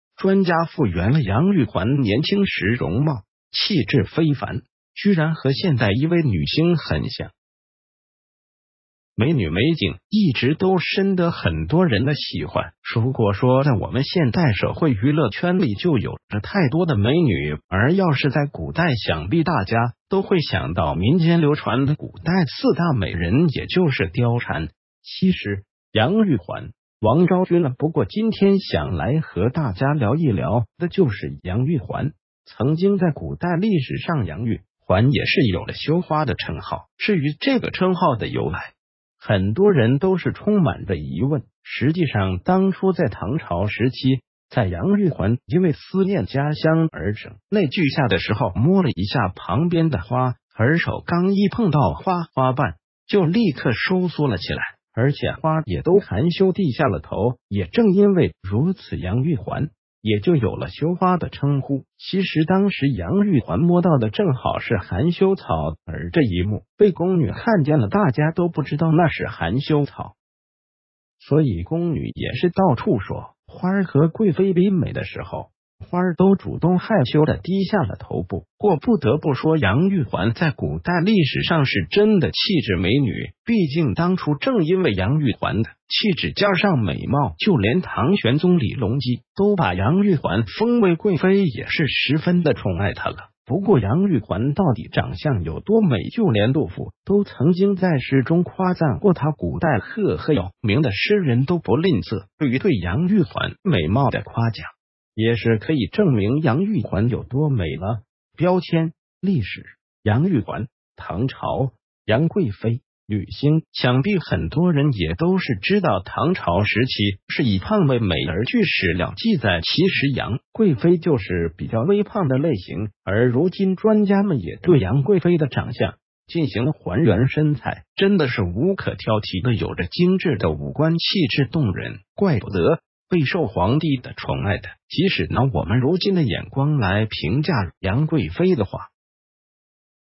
专家复原了杨玉环年轻时容貌，气质非凡，居然和现代一位女星很像
美女美景，一直都深得很多人的喜欢，如果说在我们现代社会，娱乐圈里就有着太多的美女，而要是在古代，想必大家都会想到民间流传的古代四大美人，也就是貂蝉、西施、杨玉环、王昭君了。不过今天想来和大家聊一聊的就是杨玉环。曾经在古代历史上杨玉环也是有着羞花的称号。至于这个称号的由来，很多人都是充满着疑问。实际上当初在唐朝时期，在杨玉环因为思念家乡而声泪俱下的时候，摸了一下旁边的花。而手刚一碰到花，花瓣就立刻收缩了起来，而且花儿也都含羞地下了头。也正因为如此，杨玉环也就有了羞花的称呼。其实当时杨玉环摸到的正好是含羞草。而这一幕被宫女看见了，大家都不知道那是含羞草，所以宫女也是到处说花儿和贵妃比美的时候，花儿都主动害羞地低下了头。不过不得不说，杨玉环在古代历史上是真的气质美女，毕竟当初正因为杨玉环的气质加上美貌，就连唐玄宗李隆基都把杨玉环封为贵妃，也是十分的宠爱她了。不过杨玉环到底长相有多美？就连杜甫都曾经在诗中夸赞过她，古代赫赫有名的诗人都不吝啬于对杨玉环美貌的夸奖，也是可以证明杨玉环有多美了。    标签  历史  杨玉环  唐朝  杨贵妃  女星      想必很多人也都是知道，唐朝时期是以胖为美，而据史料记载，其实杨贵妃就是比较微胖的类型。而如今专家们也对杨贵妃的长相进行了还原，身材真的是无可挑剔的，有着精致的五官，气质动人，怪不得备受皇帝的宠爱的。即使拿我们如今的眼光来评价杨贵妃的话，那也算得上是绝代美人了。话说，有很多网友还评论说杨贵妃长得和我们现代的一位女星很像，而且这位女星曾经扮演过杨贵妃的角色，她就是冯宝宝。大家觉得他们两个人像么？    标签  历史  杨玉环  唐朝  杨贵妃  女星